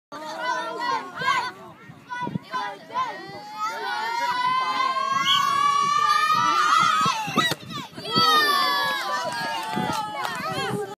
Dylan penalty 3-3-2011
Na de gewonnen wedstrijd (4-2) SVW -F5 tegen Kolping Boys F7